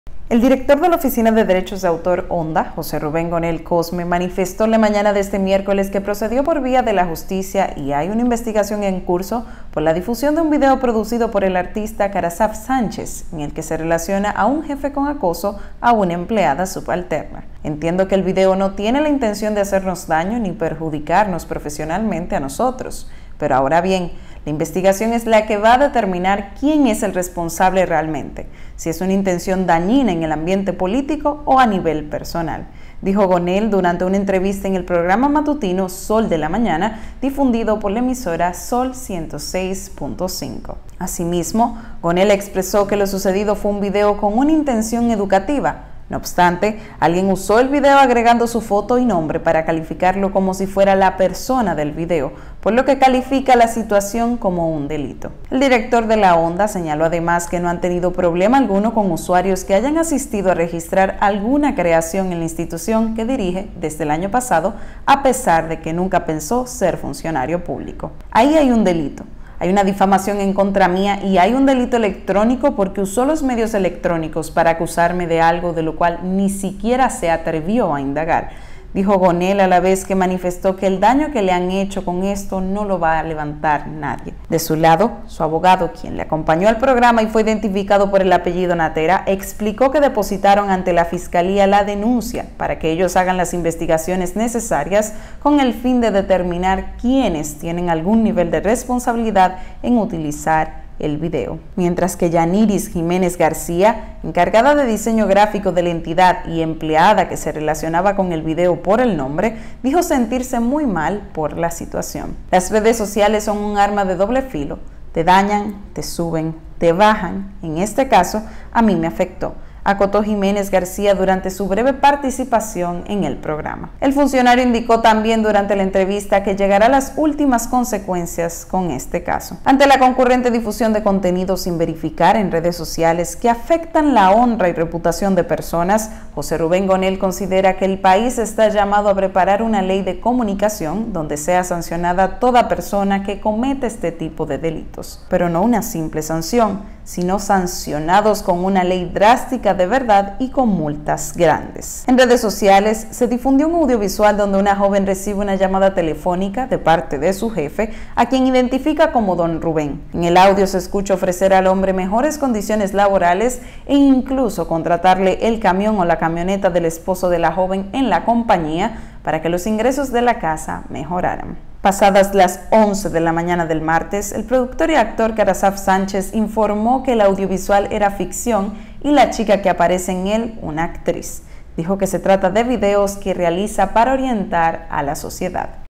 Director de ONDA dice llegará a las últimas consecuencias tras ser relacionado con video viral
El director de la Oficina de Derecho Autor (ONDA), José Rubén Gonell Cosme, manifestó la mañana de este miércoles que procedió por vía de la justicia y hay una investigación en curso por la difusión de un video producido por el artista Carrasaf Sánchez, en el que se relaciona a un jefe con acoso a una empleada subalterna.

“Entiendo que el video no tiene la intención de hacernos daño ni perjudicarnos profesionalmente a nosotros; pero ahora bien, la investigación es la que va a determinar quién es el responsable realmente, si es una intención dañina en el ambiente político o a nivel personal”, dijo Gonell durante una entrevista en el programa matutino Zol de la Mañana, difundido por la emisora ZOL 106.5.

Asimismo, Gonell expresó que lo sucedido fue un video con una intención educativa; no obstante, alguien usó el video agregando su foto y nombre para calificarlo como si fuera la persona del video, por lo que califica la situación como un delito.